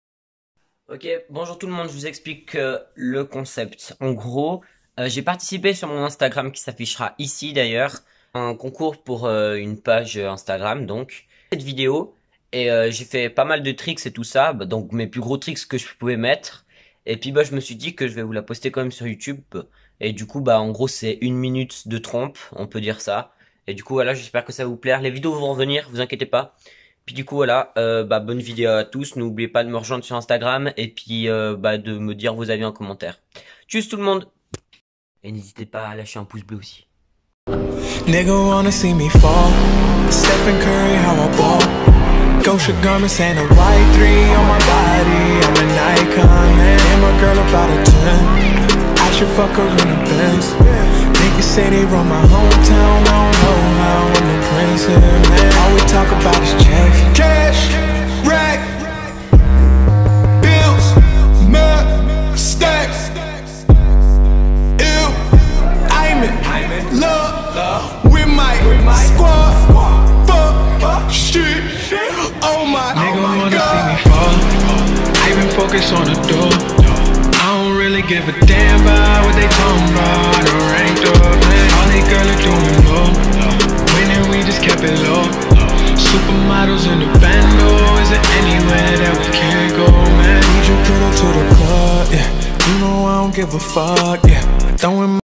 1 MINUTE DE TRAMPOLINE ! {Best of tricks}
Bonjour tous le monde, sa fesait longtemps, mais bon je reviens et je commence tranquille avec une petite edit que j'avais mis sur mon Instagram pour ma participation a 1minute2extrem donc voié  Jespere que sa vous plaira

N'hésitez pas en commentaire a me dire vos avis et a partager en masse la vidéo
ON essaie d'aller au 400 abos le plus vite possible

Bisous a bientôt ❤️